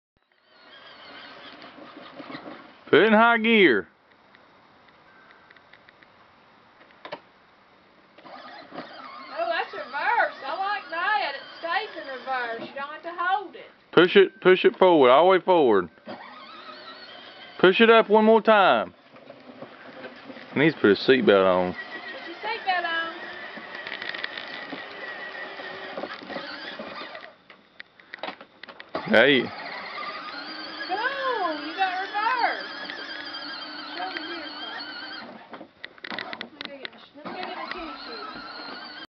Dune Racer Christmas Present
We got this for our 2 sons for Christmas, but had to return it cause the rear wheel gears slipped at times.